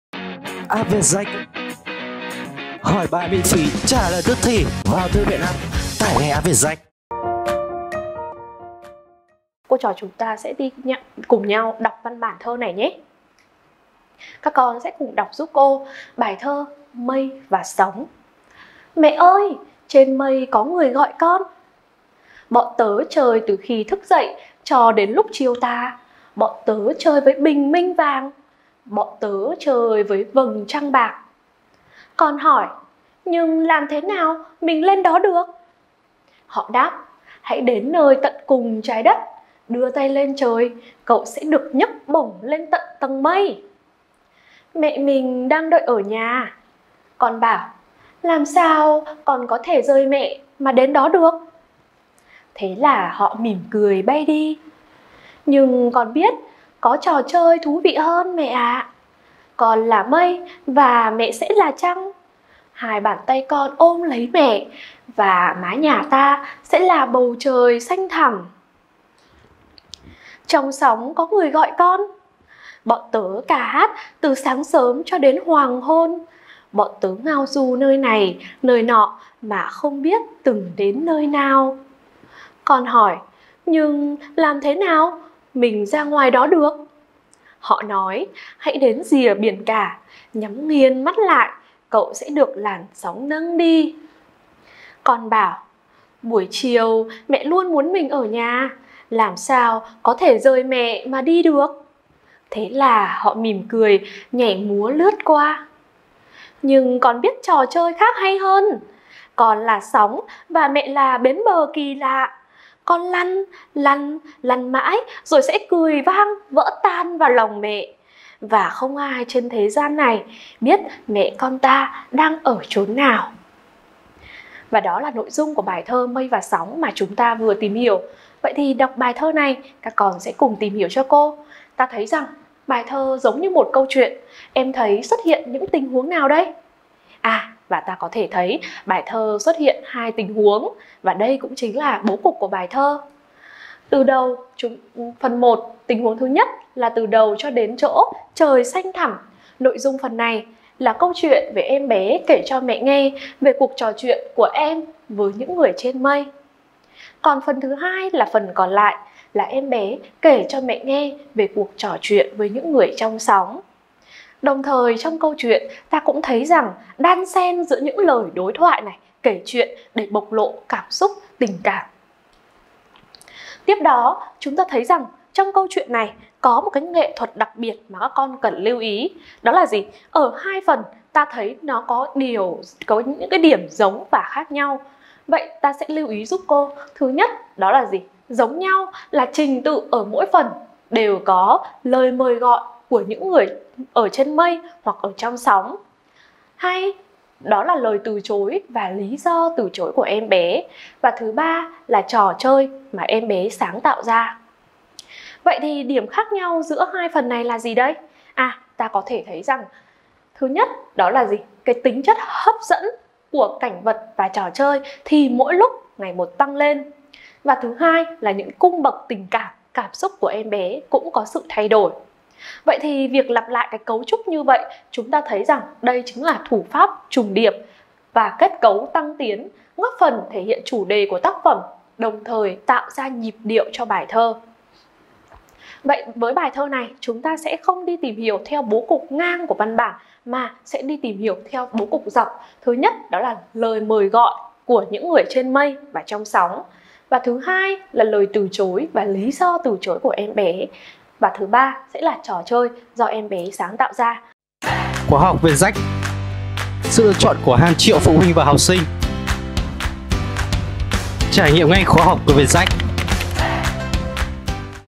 Đọc thơ Mây và sóng - Cô Phạm Lan Anh - Ngữ văn 9
🔖 Đăng ký mua sách luyện thi vào 10 của VietJack tại
Ngữ văn 9 - Mây và sóng

Video hôm nay, cô sẽ hướng dẫn các em đọc văn bản Mây và sóng của tác giả R. Ta-go trong chương trình Ngữ văn 9. Qua đó, cô cùng các em sẽ cảm nhận những nét đặc sắc trong tác phẩm. Từ đó chúng ta càng thấy hiểu sâu sắc hơn về giá trị nhân văn, nhân đạo của bài thơ. Chú ý theo dõi bài giảng của cô nhé !